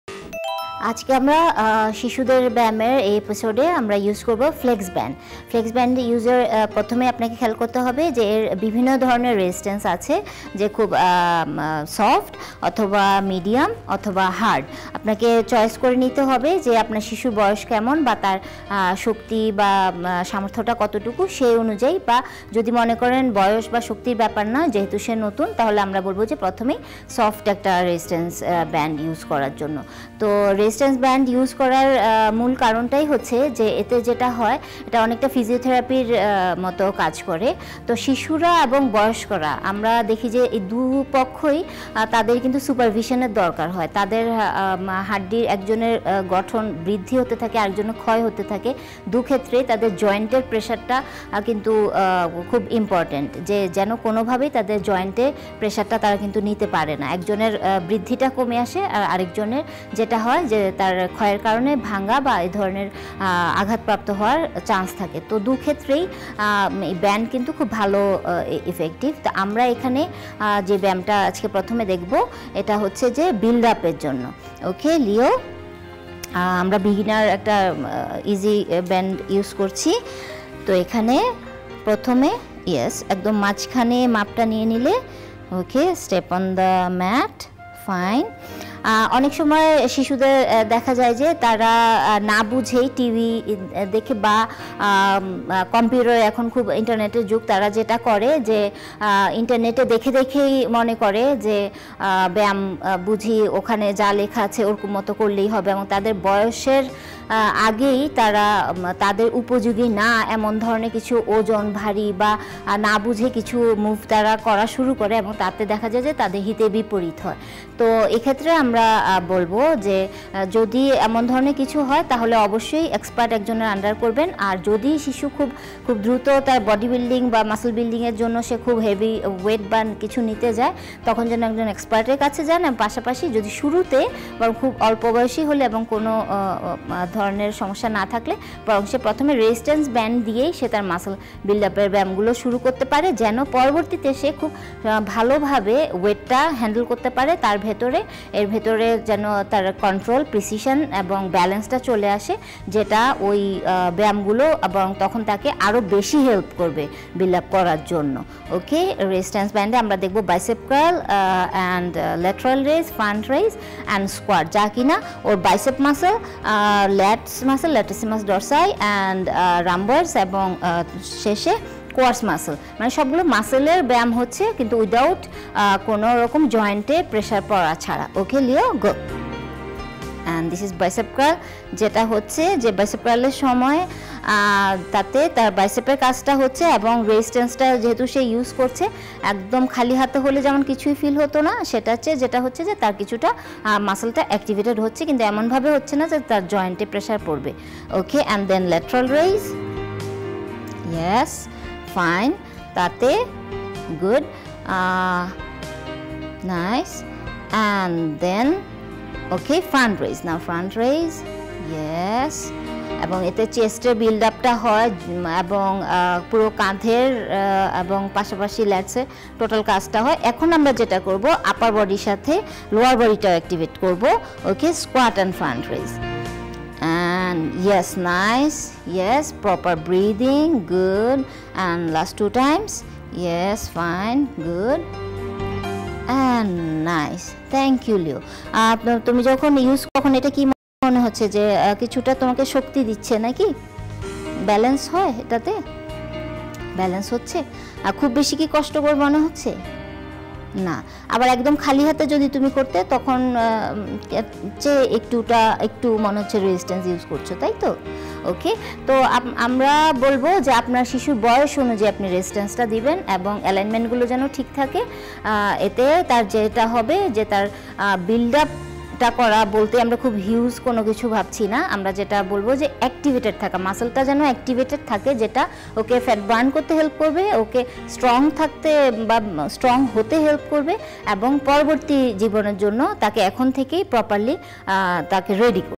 Bicep Curl, Squat with Lateral and Front Raise using FleX Band
Instructor:  Shamima Akhtar Tulee
Model: Tahseen Shaan Leon
Place: COMBAT GYM
Benefits: Activates Bicep, legs, shoulder muscles with precision.